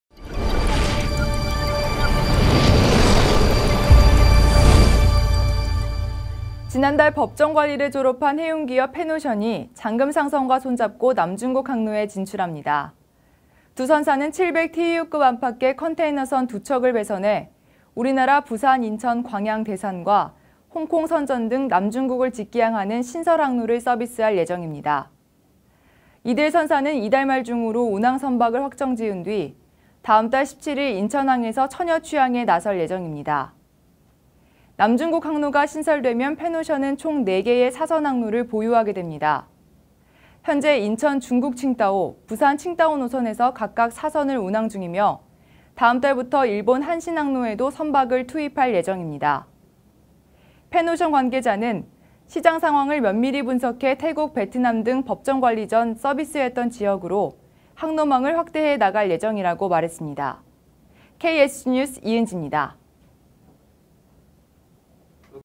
팬오션·장금상선, 남중국항로 내달 물살
지난달 법정관리를 졸업한 해운기업 팬오션이 장금상선과 손잡고 남중국항로에 진출합니다.

두 선사는 700TEU급 안팎의 컨테이너선 2척을 배선해 우리나라 부산 인천 광양 대산과 홍콩, 선전 등 남중국을 직기항하는 신설항로를 서비스할 예정입니다.

이들 선사는 이달 말 중으로 운항선박을 확정 지은 뒤 다음달 17일 인천항에서 처녀취항에 나설 예정입니다.

남중국항로가 신설되면 팬오션은 총 4개의 사선항로를 보유하게 됩니다. 현재 인천- 중국 칭다오, 부산-칭다오 노선에서 각각 사선을 운항 중이며 다음달부터 일본 한신 항로에도 선박을 투입할 예정입니다.

팬오션 관계자는 "시장상황을 면밀히 분석해 태국 베트남 등 법정관리 전 서비스해왔던 지역으로 항로망을 확대해 나갈 예정"이라고 말했습니다.

사진 : 팬오션 장금상선 로고.JPG